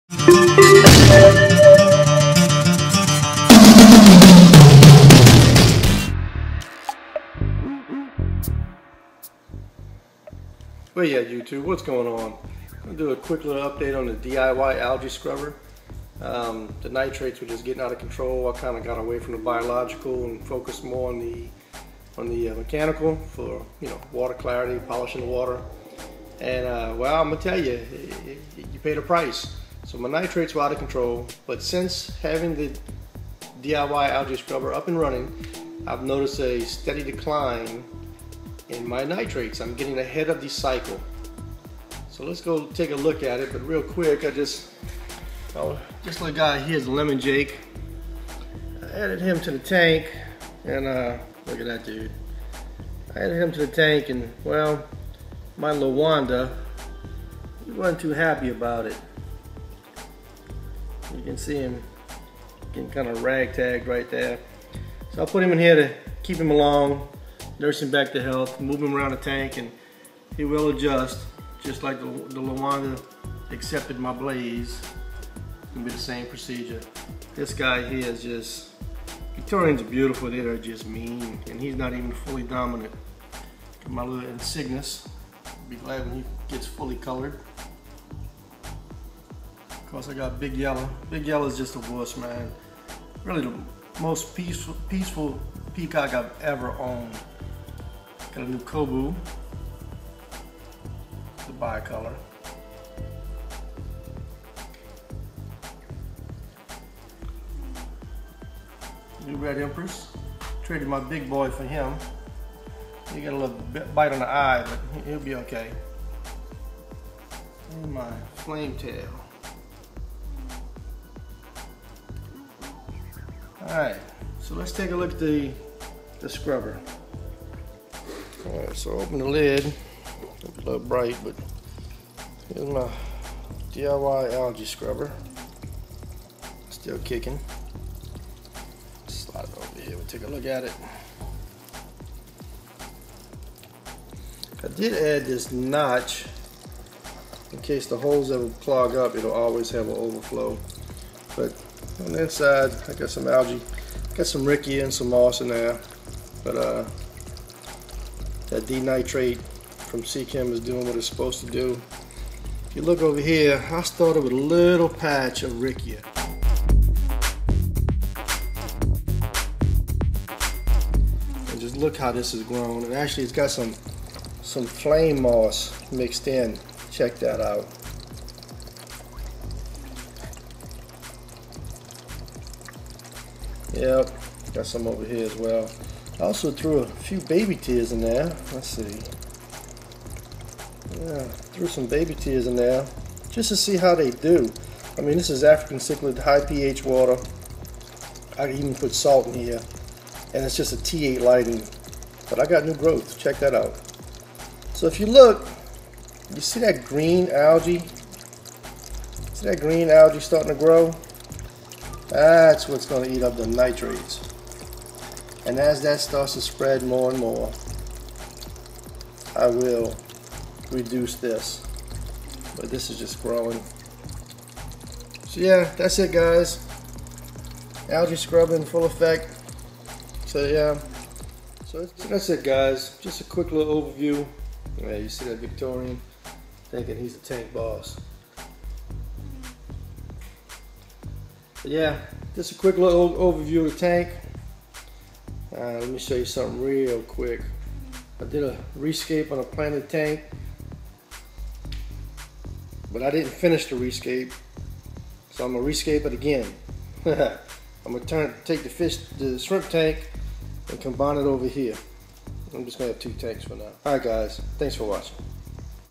Nitrate Reducer - How To- DIY Update
It's been just over 3 weeks. Some nice green algea is forming and a few other plants are doing surprisingly well. The nitrates are slowly but surely decreasing. I am pulling ahead of the nitrate production rate.